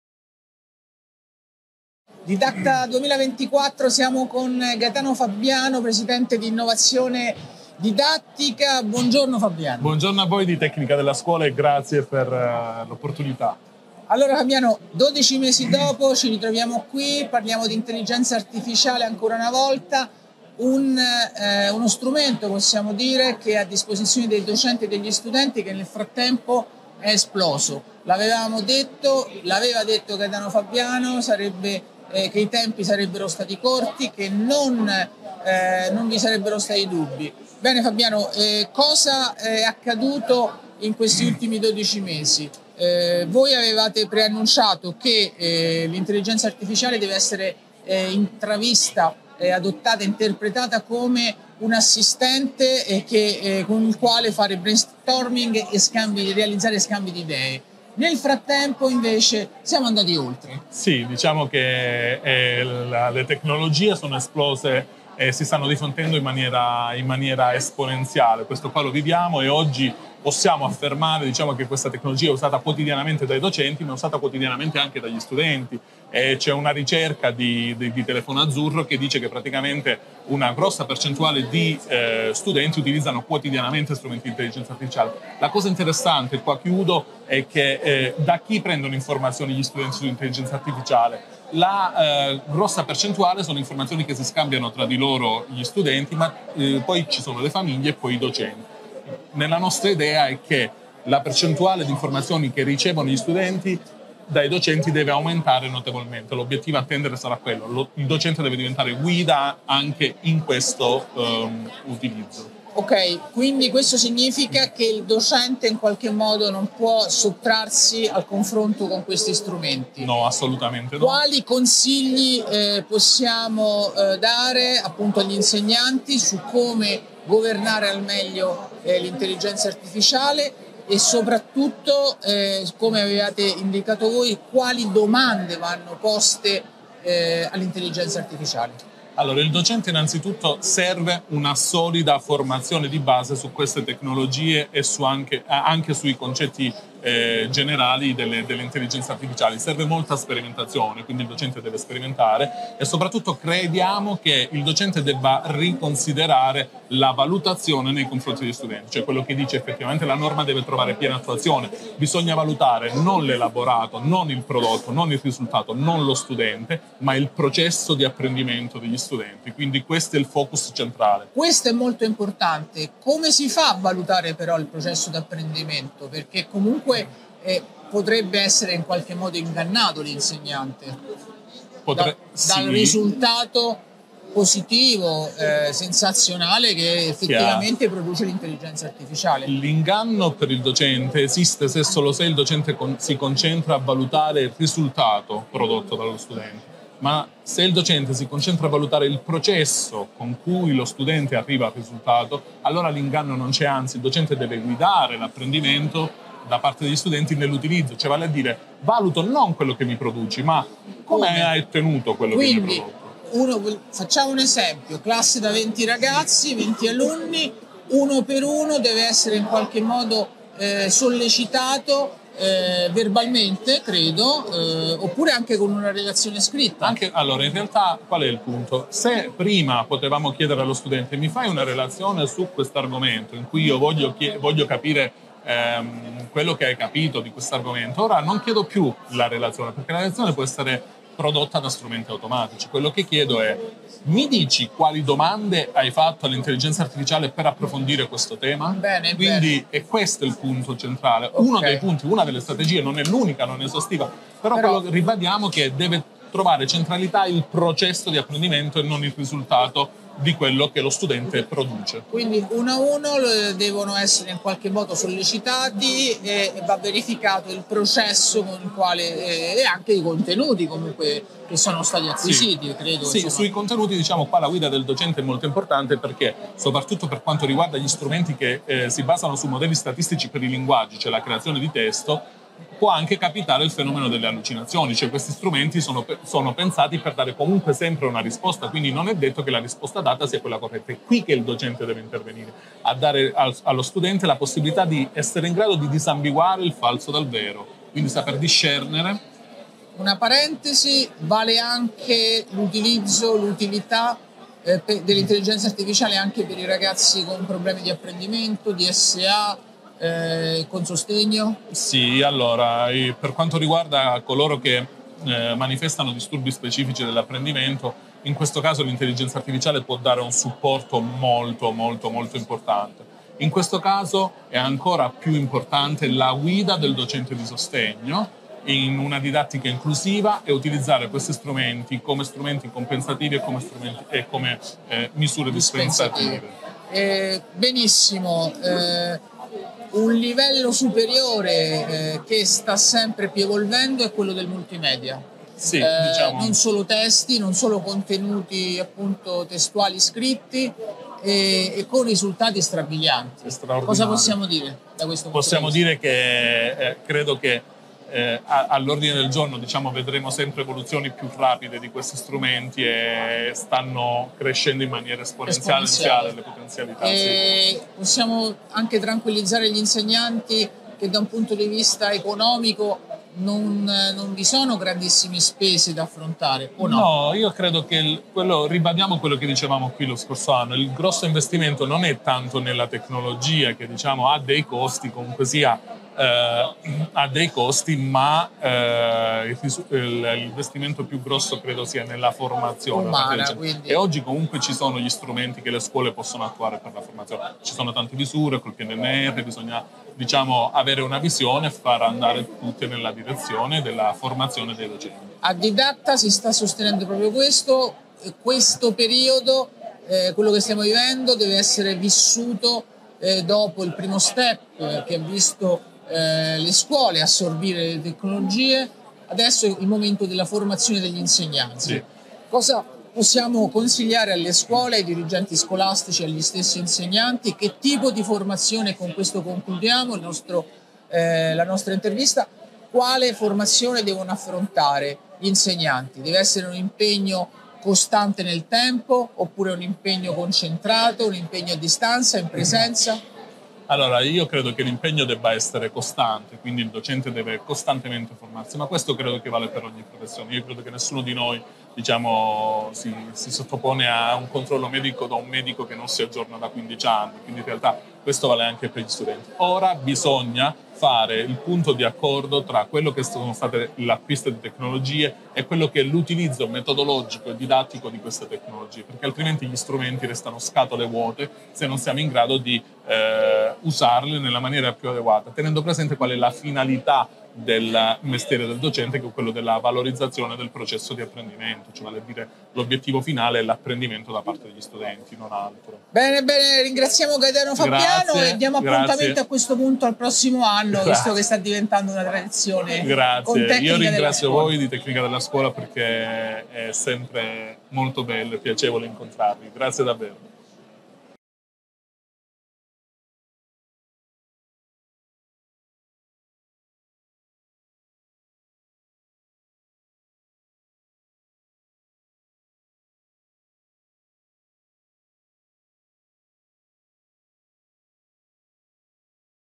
Intelligenza artificiale, Fabiano (Ai4Edu): inganna docenti se valutano i risultati e non i processi
scuola docenti tecnicadellascuola Didacta Italia 2024 ha avuto il via. Dal 20 al 22 marzo, il più importante appuntamento fieristico sull’innovazione del mondo della scuola, apre le sue porte a Fortezza Da Basso, Firenze. Per l'occasione, ai microfoni della Tecnica della Scuola, Gaetano Fabiano co-fondatore Ai4Edu, ci ha parlato di intelligenza artificiale a scuola e valutazione.

e sui nostri canali social